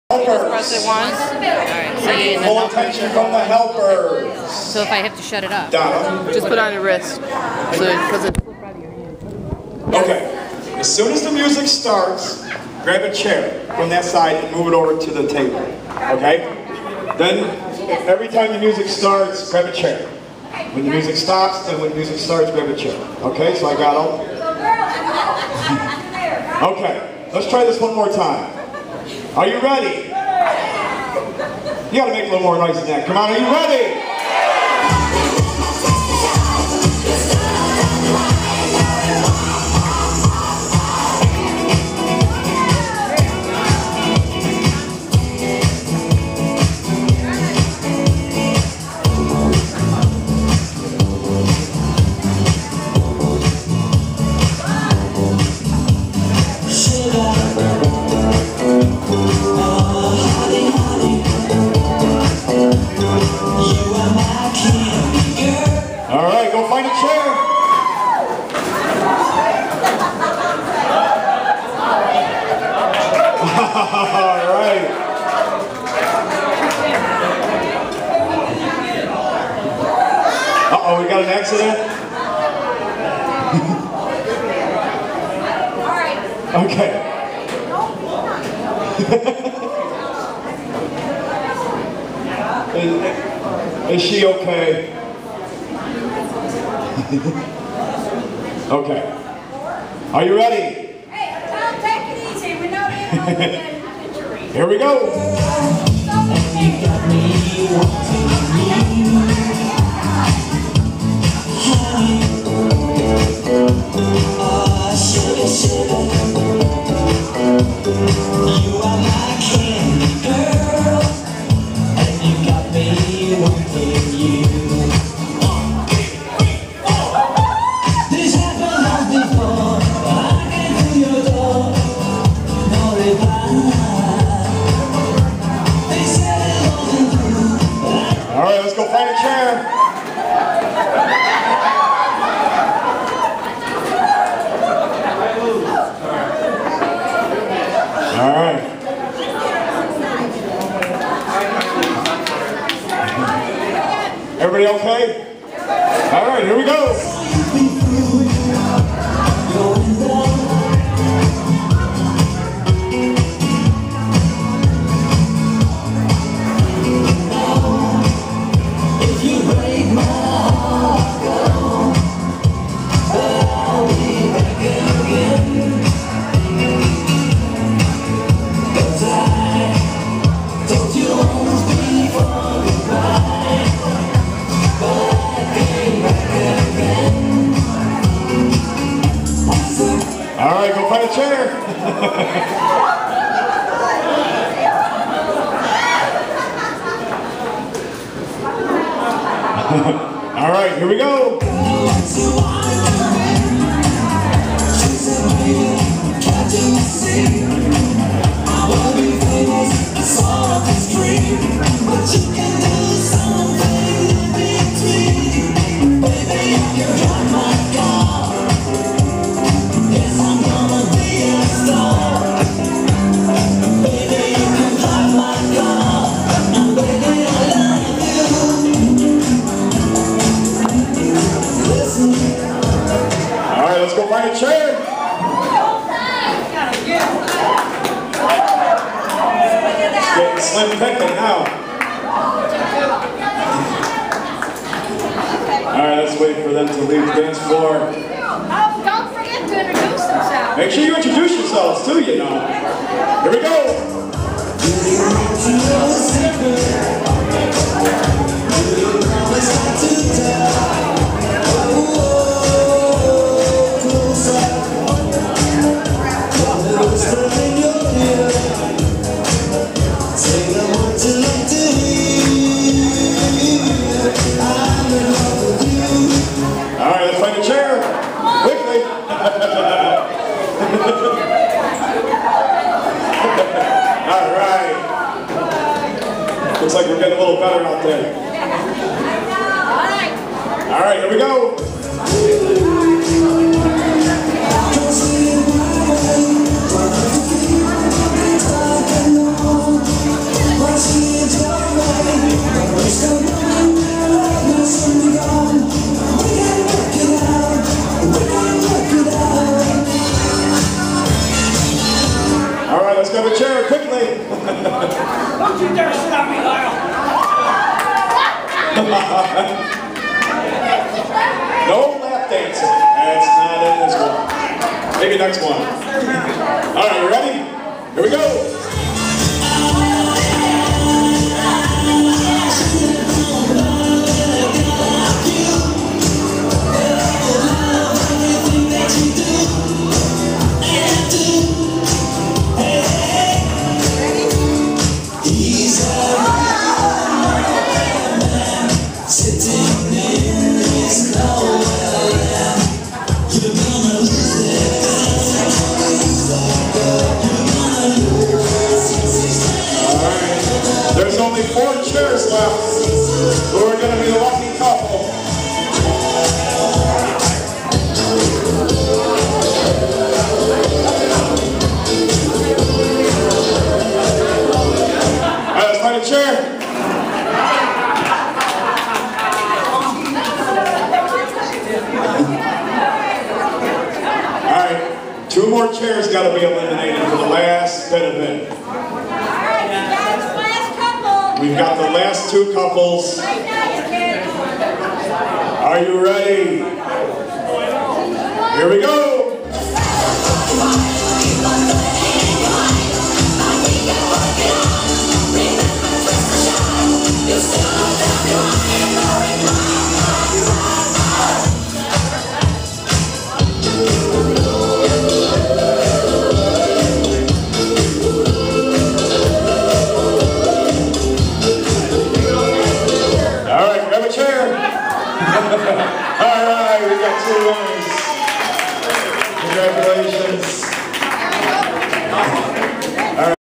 line dance mixer
9/29/13 dance @ us marine cops in Ilion, NY